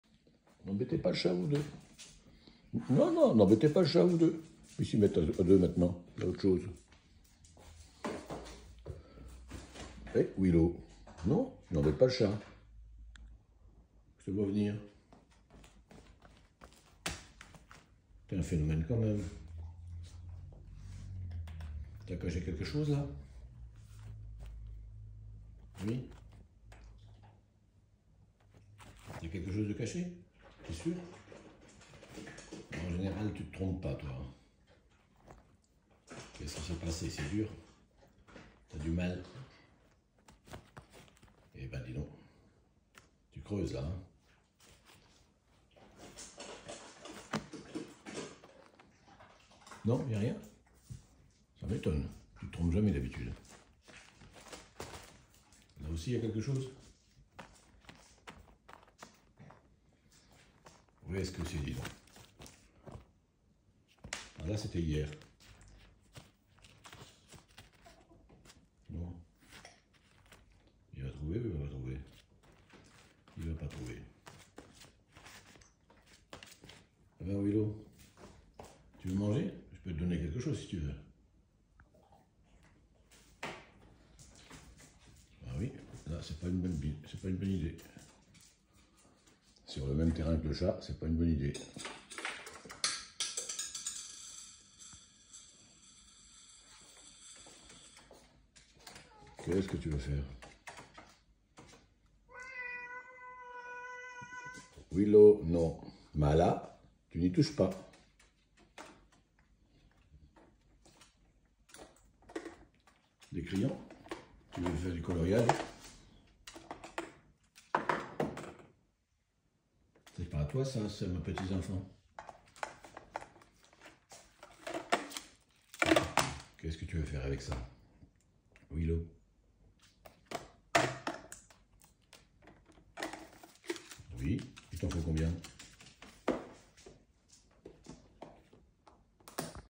réveil en fanfare
Willow réclame pour entrer dans la maison et jouer avec ses compagnons